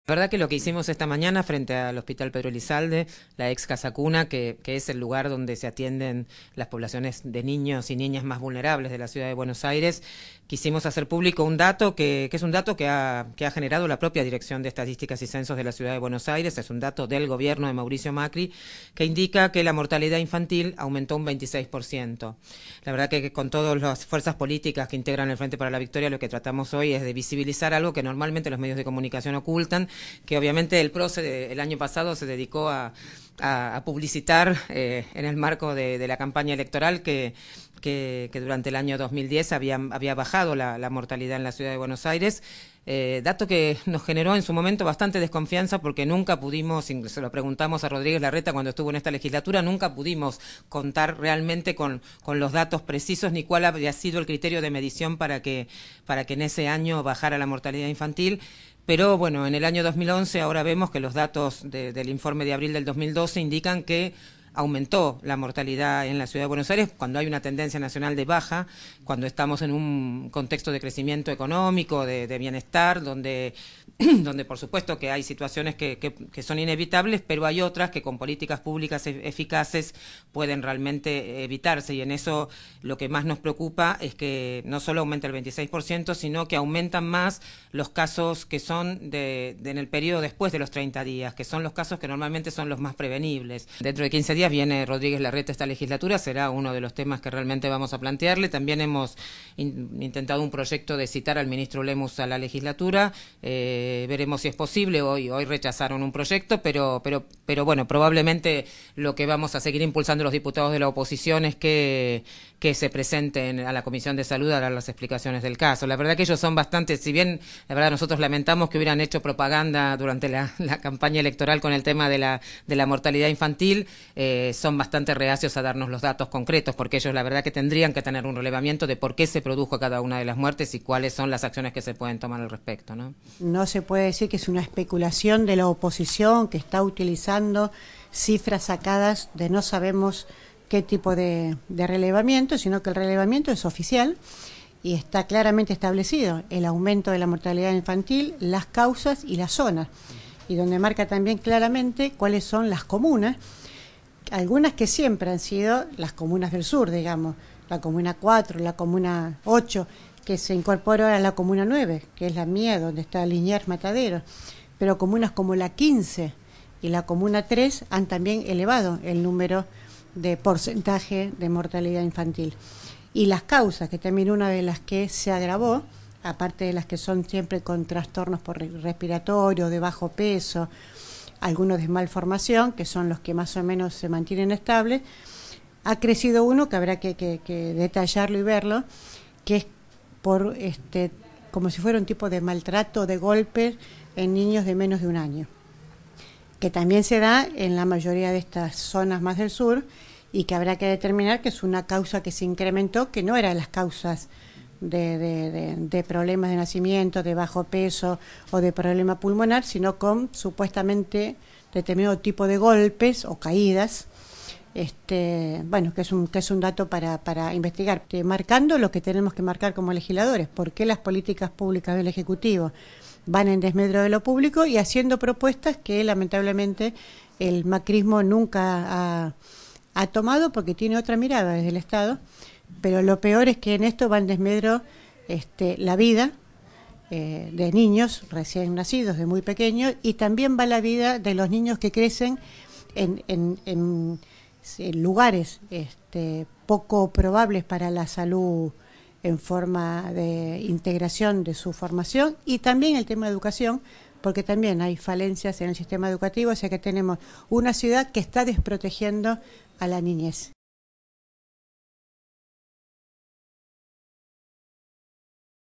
Denuncian que en la Ciudad la mortalidad infantil creció un 26 por ciento
Legisladores, funcionarios y representantes de distintas organizaciones kirchneristas de la ciudad de Buenos Aires denunciaron hoy que la mortalidad infantil en el distrito "aumentó un 26 por ciento".  Leer más
Seguinos: